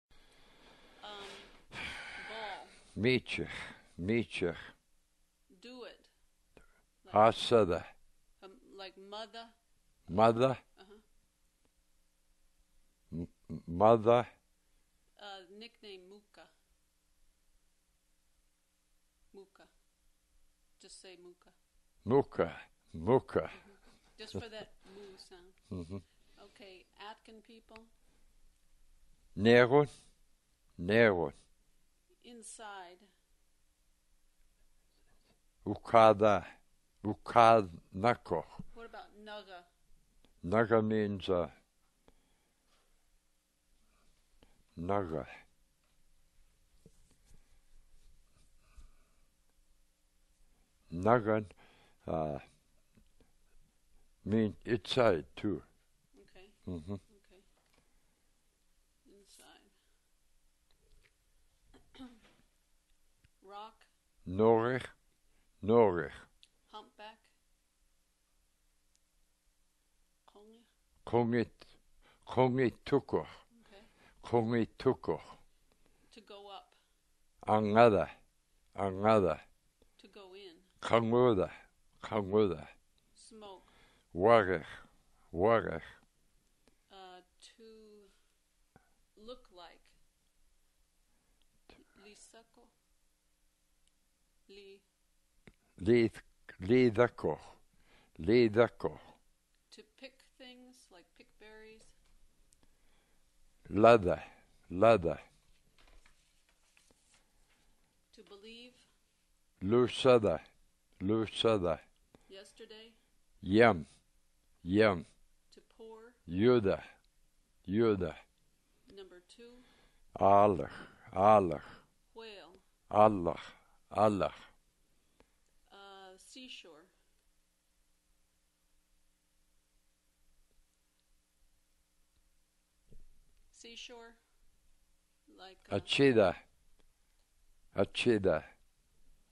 Aleut audio: ale_eastern_word-list_1996_48
Aleut (ale) audio: ale_eastern_word-list_1996_48
Language family: Eskimo-Aleut; Aleut;
St. George Island, Pribiloff Islands, Alaska

Description: Language Aleut (Eastern dialect).  ale_eastern_word-list_1996_01.html, entries: 54 - 73

Contributor: Andronik Kashevarof
UCLA Phonetics Archive